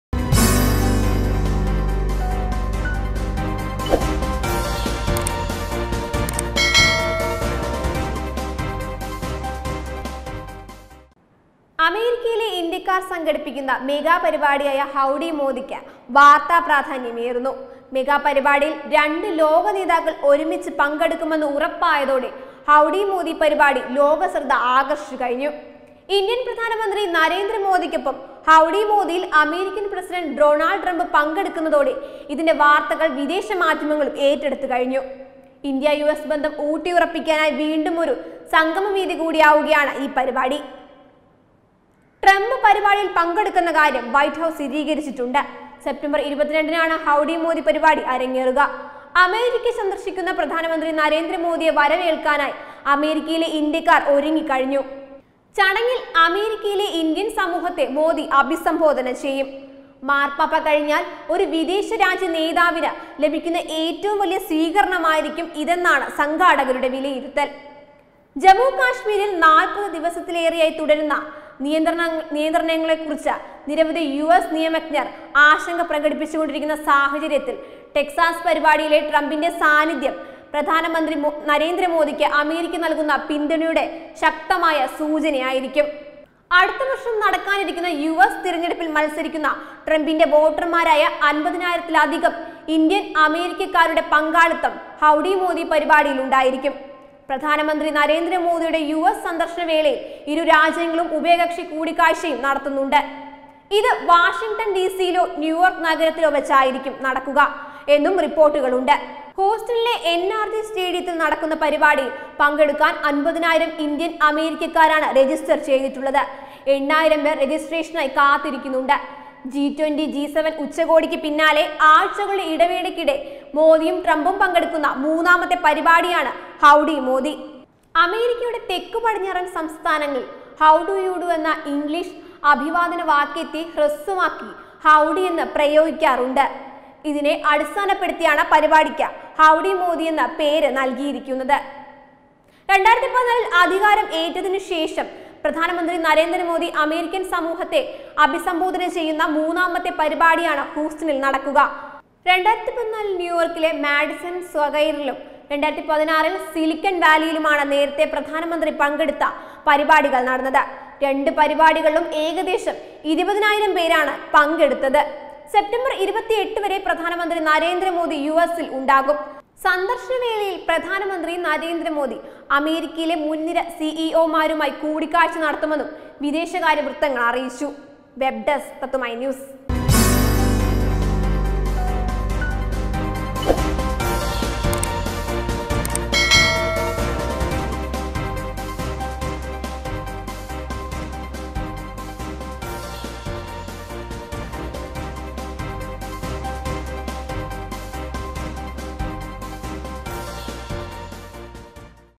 മാര്‍പാപ്പയ്ക്ക് ശേഷം മോദി..!! "ഹൗഡി മോദി"യെ നെ‍ഞ്ചോട് ചേര്‍ത്ത് യു എസ്
അമേരിക്കയിലെ ഇന്ത്യക്കാര്‍ സംഘടിപ്പിക്കുന്ന മെഗാപരിപാടിയായ ഹൗഡി മോദിക്ക് വാര്‍ത്താ പ്രാധാന്യം ഏറുന്നു. മെഗാ പരിപാടിയില്‍ രണ്ട് ലോക നേതാക്കള്‍ ഒരുമിച്ച് പങ്കെടുക്കുമെന്ന് ഉറപ്പായതോടെ  ഹൗഡി മോദി പരിപാടി ലോക ശ്രദ്ധ ആകര്‍ഷിച്ചുകഴിഞ്ഞു.ഇന്ത്യന്‍ പ്രധാനമന്ത്രി നരേന്ദ്ര മോദിക്കൊപ്പം ഹൗഡി മോദി'യില്‍ അമേരിക്കൻ പ്രസിഡന്റ് ഡൊണാള്‍ഡ് ട്രംപ് പങ്കെടുക്കുന്നതോടെ ഇതിന്‍റെ വാര്‍ത്തകള്‍  വിദേശ മാധ്യമങ്ങളും ഏറ്റെടുത്തിട്ടുണ്ട്.  ഇന്ത്യ–യു.എസ് ബന്ധം ഊട്ടിയുറപ്പിക്കാനായി വീണ്ടും ഒരു സംഗമവേദി കൂടിയാണ് ഈ മെഗാപരിപാടി.